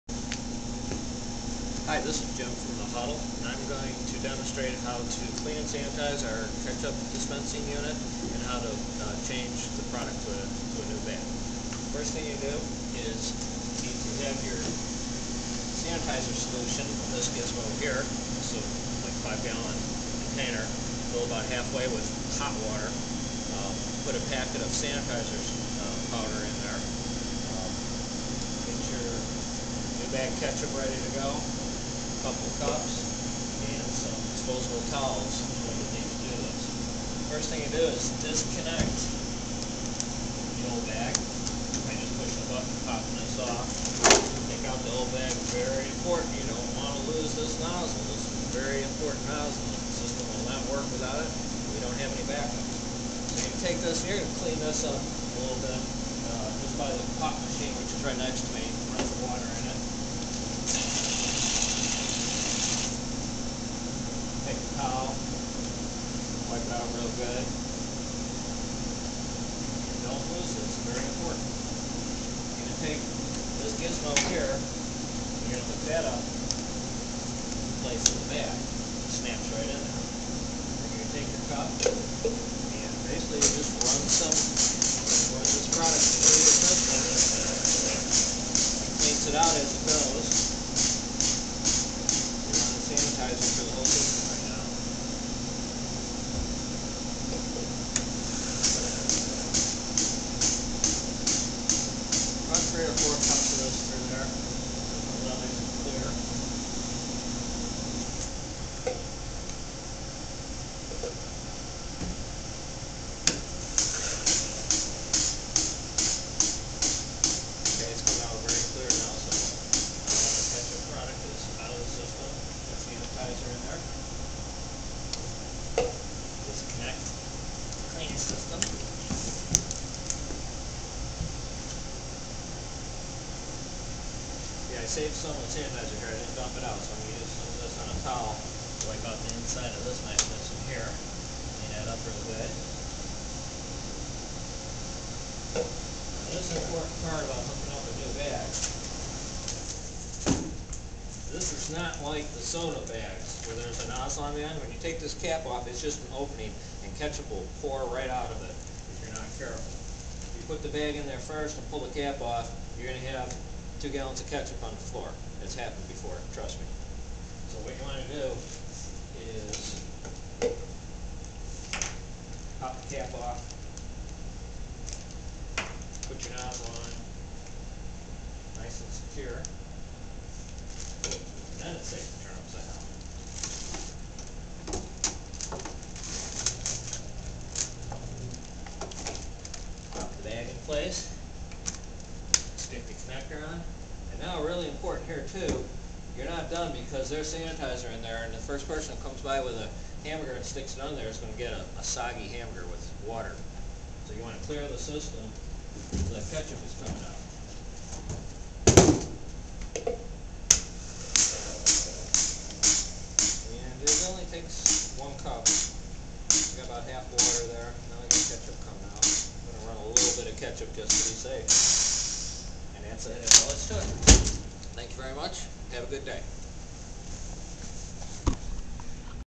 ketchup at the huddle
how to clean and change ketchup dispenser